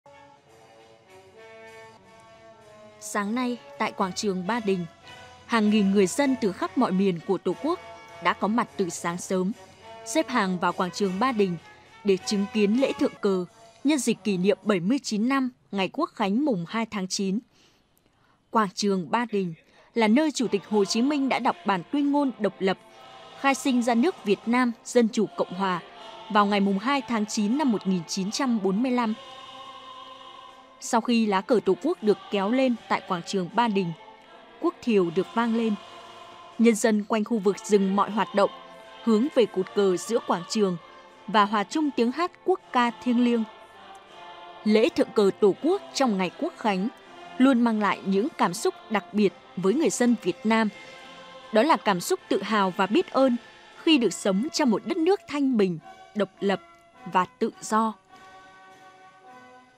Lễ thượng cờ mừng 79 năm Quốc khánh 2/9 | Tin tức
Chào mừng 79 năm ngày Quốc khánh 2/9, sáng nay, nghi lễ thượng cờ cấp quốc gia được tổ chức trọng thể tại Quảng trường Ba Đình ,Hà Nội.